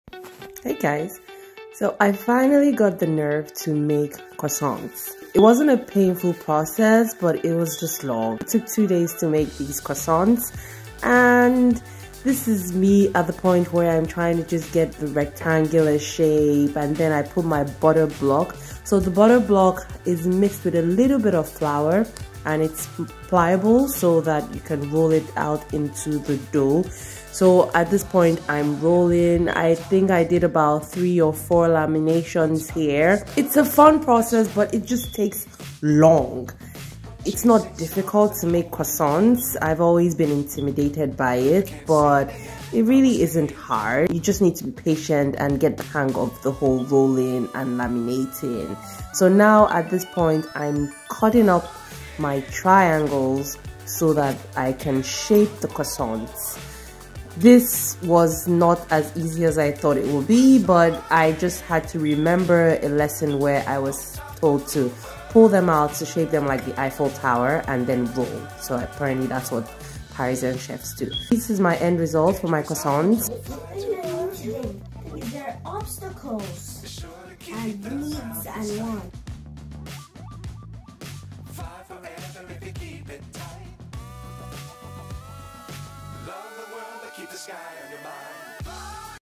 Homemade croissant recipe
Dough ingredients:

540g bread flour
180g milk
170g water
55g sugar
10g salt
5g instant yeast

Butter block:

300g butter
50g bread flour

Mix dough ingredients in a mixing bowl for about 4 mins.

Remove from bowl, shape into a rectangle.

Wrap in cling film and let it rest in the fridge overnight.

For butter block, combine butter and flour and shape into a rectangle and store in the fridge until needed.